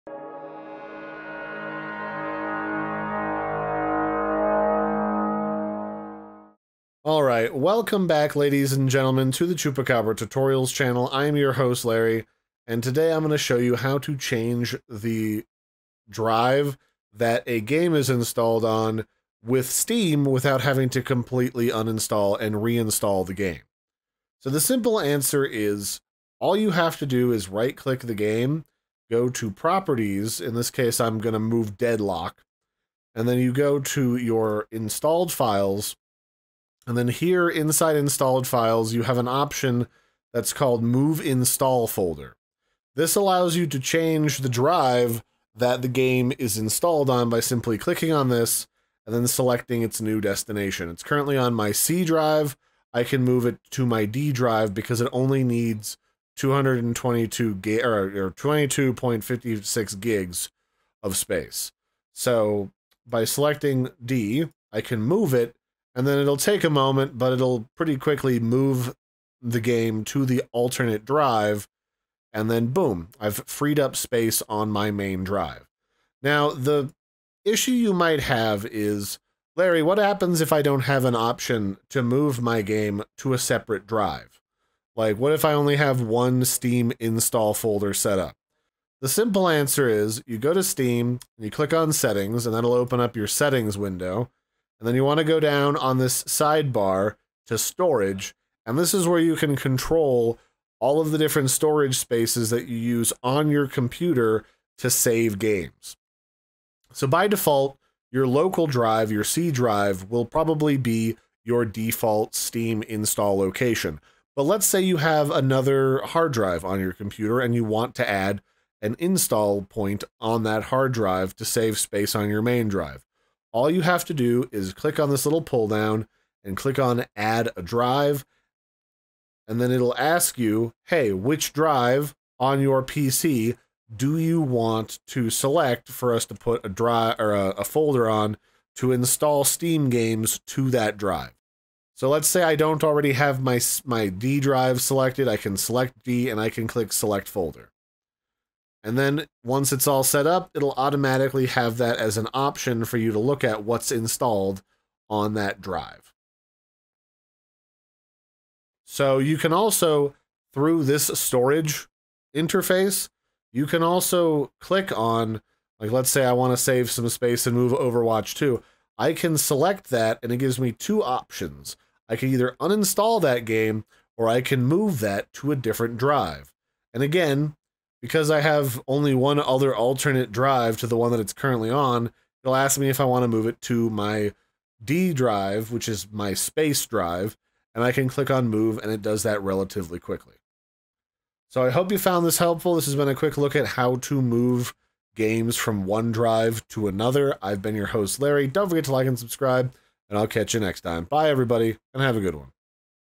How to Move INSTALLED Steam Games between DRIVES - 2024 Updated Guide
Did you know you can change the drive a steam game is installed on without uninstalling it first? You can! This quick guide will show you how you can easily create new install folder locations on your computer and move your installed steam games between drives with just a couple of button clicks. No reinstallation required. Updated for 2024.